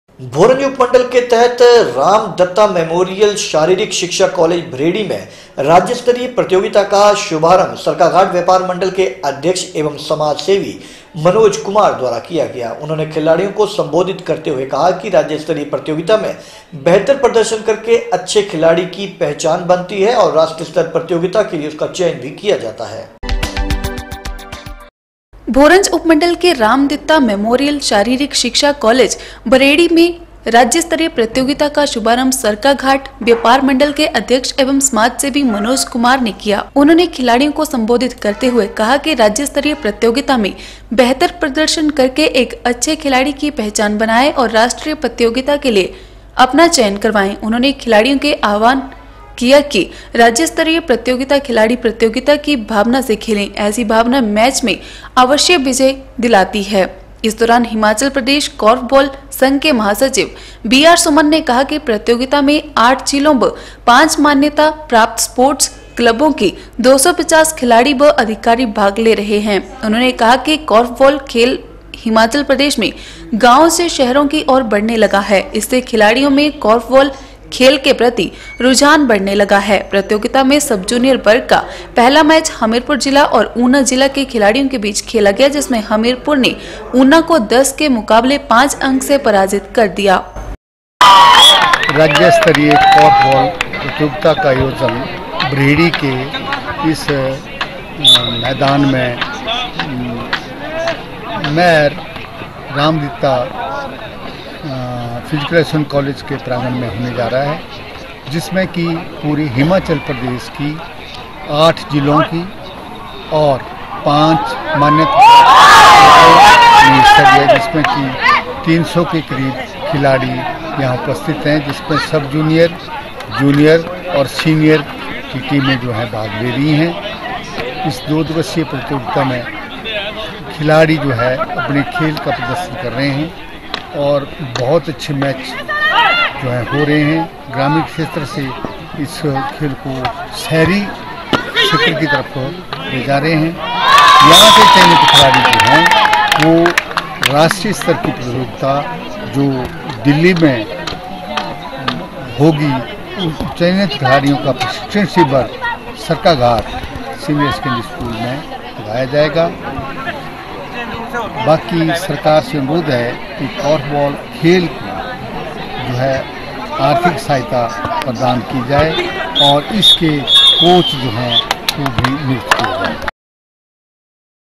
3 FEB N 6 कोेर्फबाल स्पर्धा में हमीरपुर ने उना को 5 अंक से दी शिकस्त
भोरंज उपमंडल के राम दित्ता मैमोरियल शारीरिक शिक्षा काॅलेज भरेडी में राज्य स्तरीय प्रतियोगिता का शुभारंभ सरकाघाट व्यापार मंडल के अध्यक्ष एवं समाज सेवी मनोज कुमार ने किया । उन्होंने खिलाडियों को संबोधित करते हुए कहा कि राज्य स्तरीय प्रतियोगिता में बेहतर प्रदर्शन करके अच्छे खिलाडी की पहचान बनाएं और राष्ट्रीय प्रतियोगिता के लिए अपना चयन करवाएं।